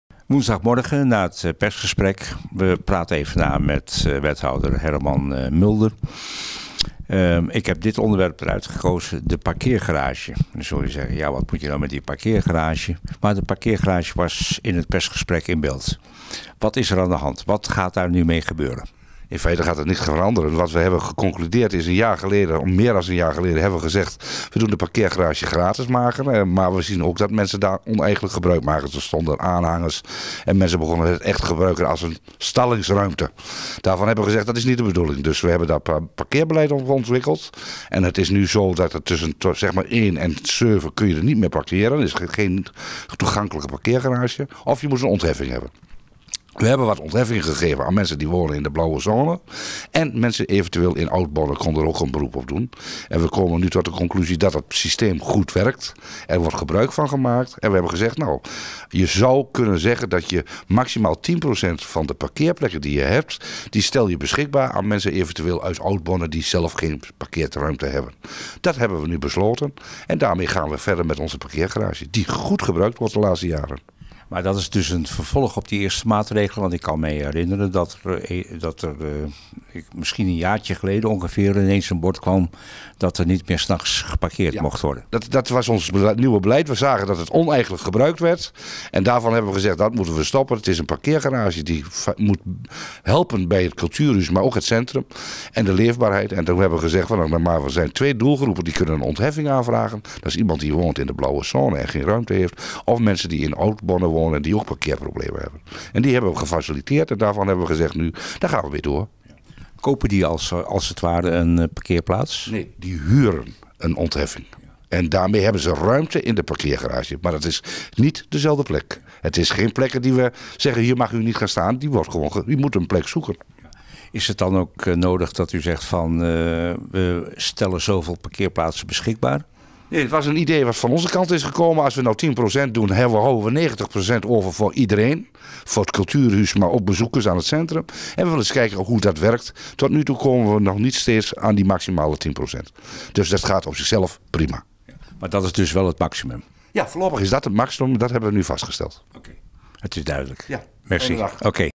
Wethouder Herman Mulder over aanpassing beleid parkeergarage
Parkeren in de parkeergarage onder het Kulturhus is van 01:00 - 07:00 's nachts verboden. Het werd teveel een stalling. Bewoners van de 'blauwe zone' konden een ontheffing krijgen.
Bewoners van Oud Borne willen dat ook wel. het beleid is nu zo aangepast dat ook zij daarvoor in aanmerking kunnen komen.
Maximaal 10% van de parkeerplaatsen is daarvoor beschikbaar.

Interview en tekst: Jan Stoop (Bornse Courant).
Camera en montage: Erik Veltkamp